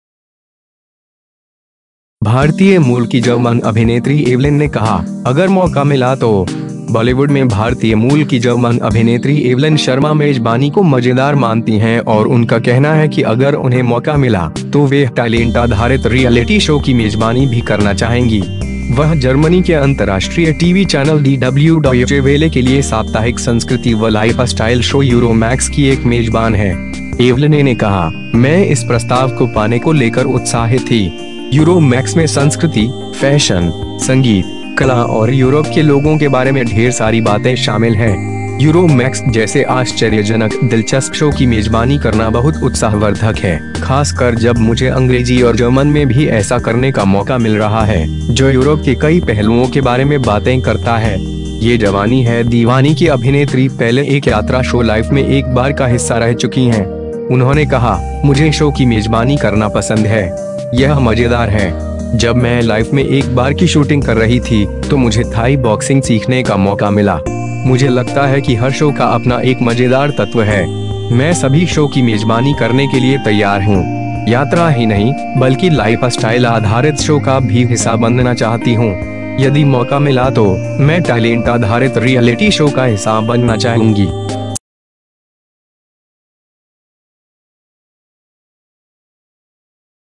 भारतीय मूल की जर्मन अभिनेत्री एवलिन ने कहा, अगर मौका मिला तो
बॉलीवुड में भारतीय मूल की जर्मन अभिनेत्री एवलिन शर्मा मेजबानी को मजेदार मानती हैं और उनका कहना है कि अगर उन्हें मौका मिला, तो वह टैलेंट-आधारित रियलिटी शो की मेजबानी भी करना चाहेंगी।

वह जर्मनी के अंतर्राष्ट्रीय टीवी चैनल डीडब्ल्यू- डॉयचे वेले के लिए साप्ताहिक संस्कृति व लाइफस्टाइल शो ‘यूरोमैक्स’ की एक मेजबान हैं।

एवलिने ने कहा, ‘‘मैं इस प्रस्ताव को पाने को लेकर उत्साहित थी। ‘यूरोमैक्स’ में संस्कृति, फैशन, संगीत, कला और यूरोप के लोगों के बारे में ढेर सारी बातें शामिल हैं। ‘यूरोमैक्स’ जैसे आश्चर्यजनक दिलचस्प शो की मेजबानी करना बहुत उत्साहवर्धक है, खासकर जब मुझे अंग्रेजी और जर्मन में भी ऐसा करने का मौका मिल रहा है, जो यूरोप के कई पहलुओं के बारे में बातें करता है।’’

‘ये जवानी है दीवानी’ की अभिनेत्री पहले एक यात्रा शो ‘लाइफ में एक बार’ का हिस्सा रह चुकी हैं।

उन्होंने कहा, ‘‘मुझे शो की मेजबानी करना पसंद है। यह मजेदार है। जब मैं ‘लाइफ में एक बार’ की शूटिंग कर रही थी, तो मुझे थाई बॉक्सिंग सीखने का मौका मिला। मुझे लगता है कि हर शो का अपना एक मजेदार तत्व है। मैं सभी शो की मेजबानी करने के लिए तैयार हूं। यात्रा ही नहीं, बल्कि लाइफस्टाइल-आधारित शो का भी हिस्सा बनना चाहती हूं। यदि मौका मिला तो, मैं टैलेंट आधारित रियलिटी शो का हिस्सा बनना चाहूंगी।’